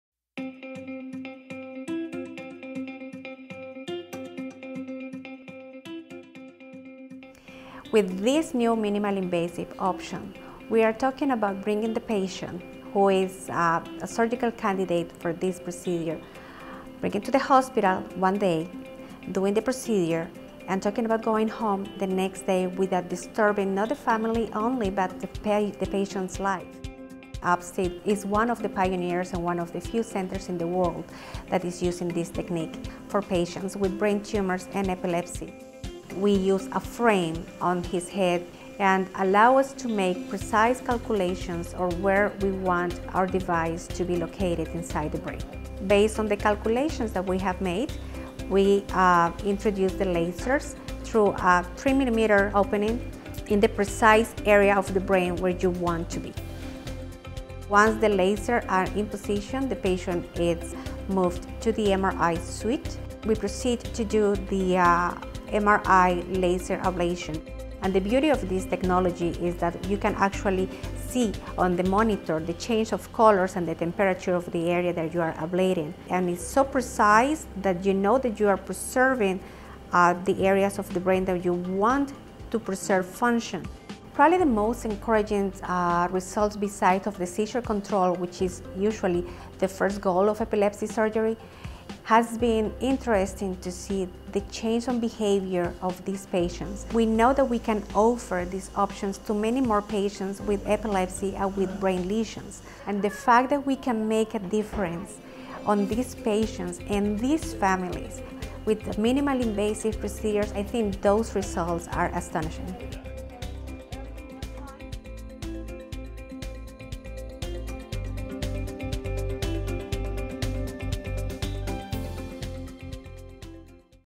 New MRI-Guided Laser Surgery for Children with Brain Tumors and Epilepsy
Dr Zulma Tovar Spinoza, Director of Pediatric Neurosurgery, discusses the new MRI-Guided Laser Surgery for children with brain tumors and epilepsy now available at Upstate Golisano Children's Hospital in Syracuse, NY. Dr Tovar Spinoza is a pioneer in this new technique and Upstate Golisano Children's Hospital is among the first in the country to offer this minimally invasive surgery that is producing encouraging results for patients and families.